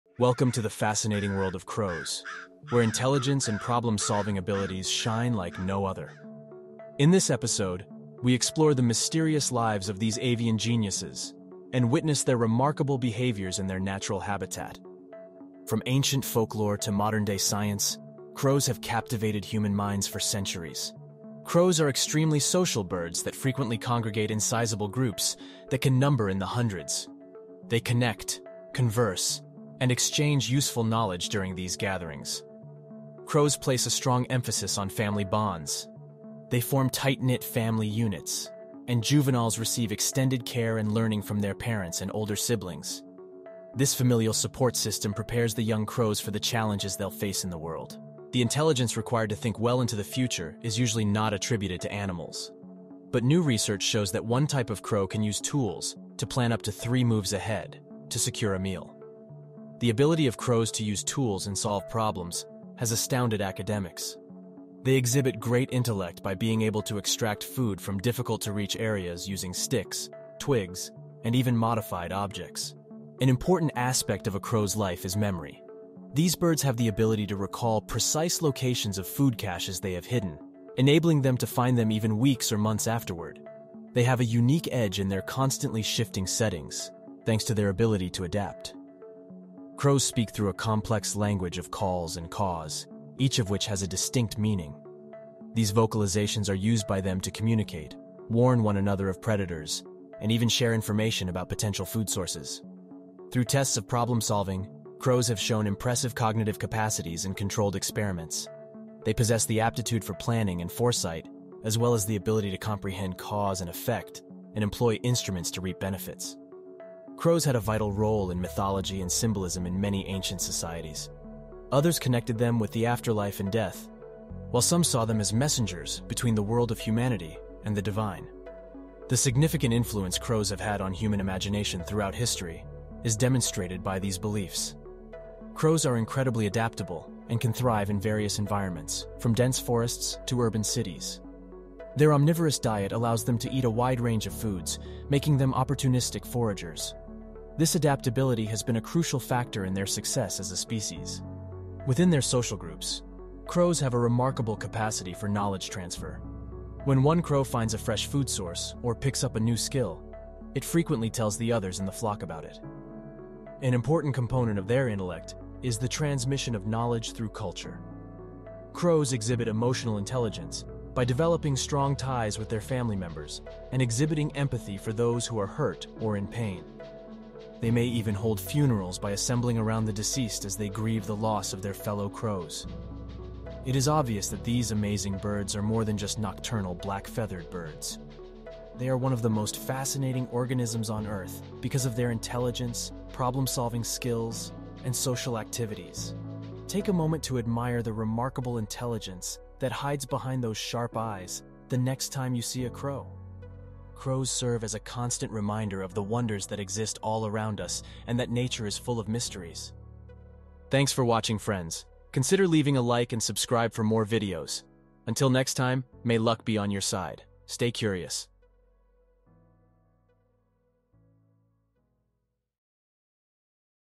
Mind-Blowing Crows: The Smartest Birds on Earth?
In this video, we delve into the fascinating world of crows and explore their extraordinary intelligence. Crows have long intrigued scientists and nature enthusiasts alike with their problem-solving abilities, social dynamics, and remarkable communication skills. Join us on this journey as we uncover the secrets behind why crows are considered one of the most intelligent species on our planet.
Crows are renowned for their problem-solving skills, which rival those of primates and dolphins. Through a series of jaw-dropping experiments and real-life encounters, we showcase how crows exhibit a wide range of cognitive abilities.